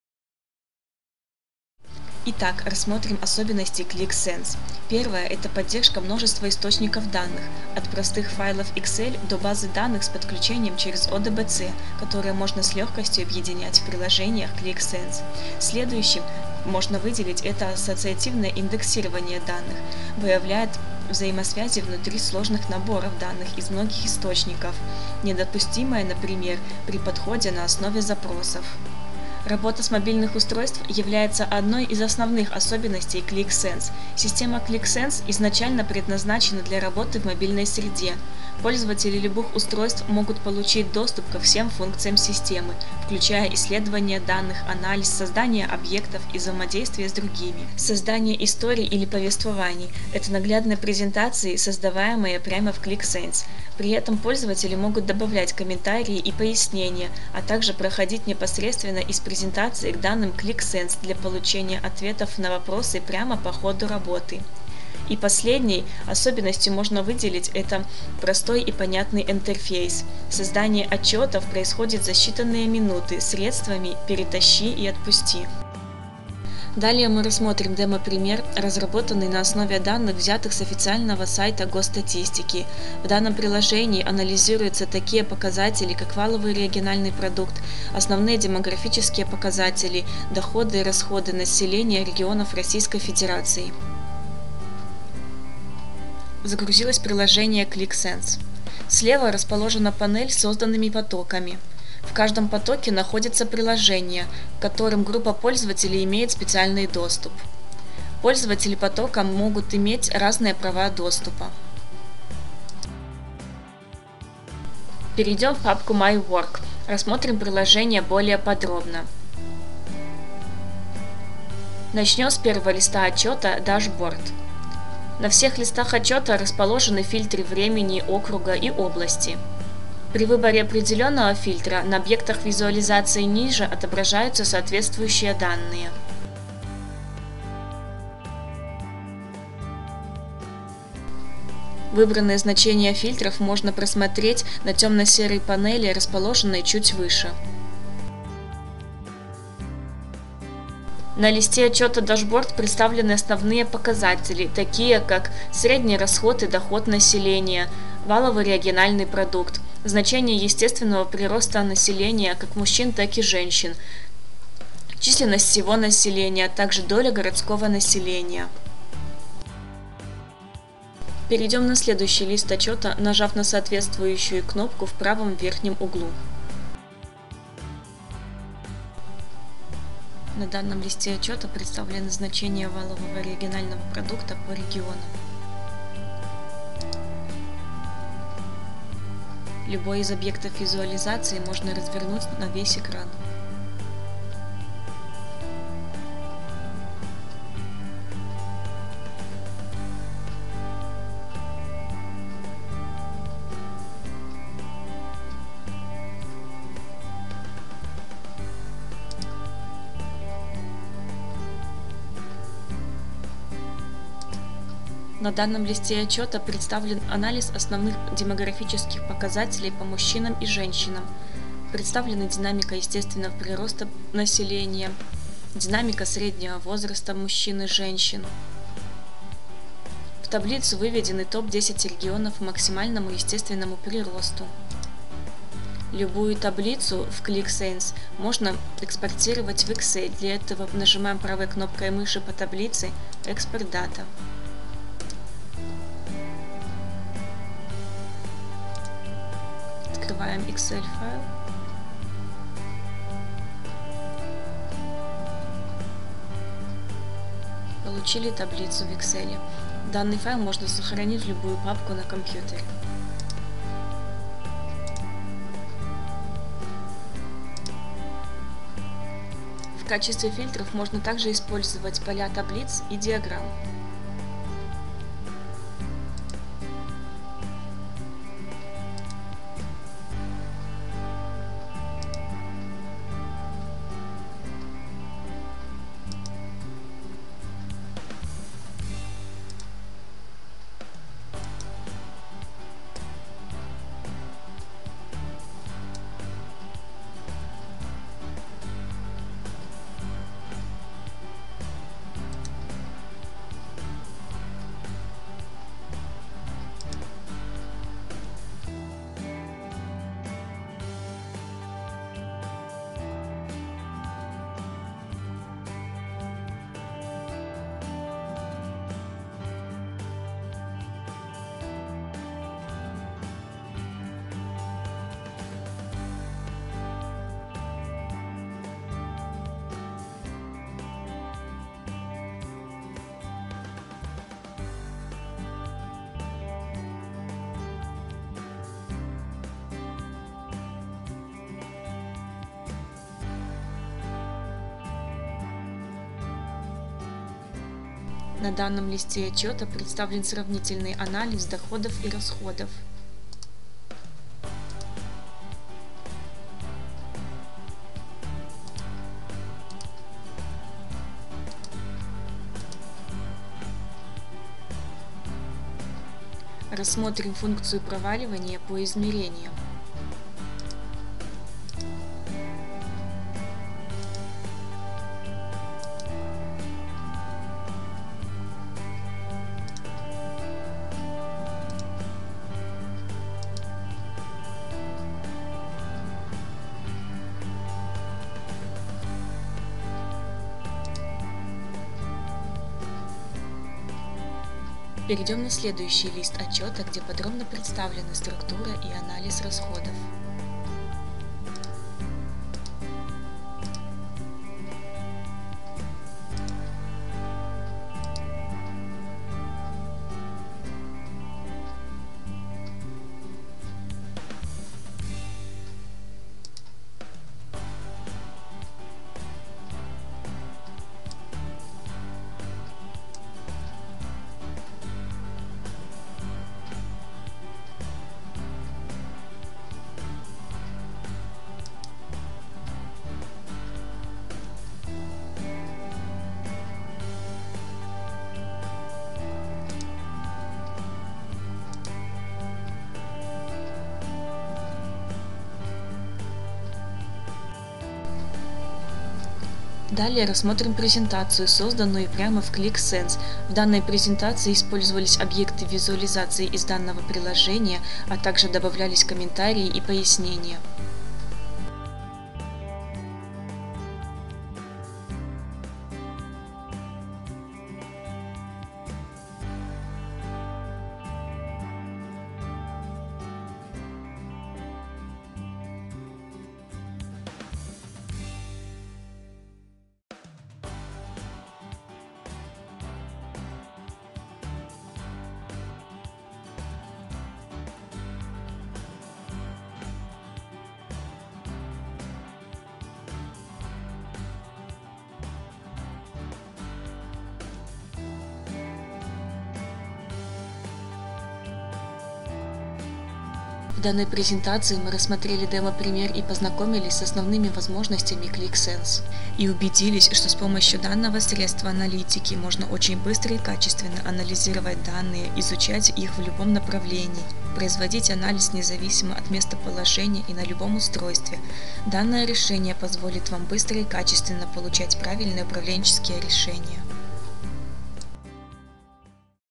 Анализ статистики Qlik Sense демографические данные в Qlik Sense обгатить данные
Хотите обогатить внутренние данные компании внешней информацией (демография, температура и т.д.)? Например для расчета потенциала продаж - получите информацию на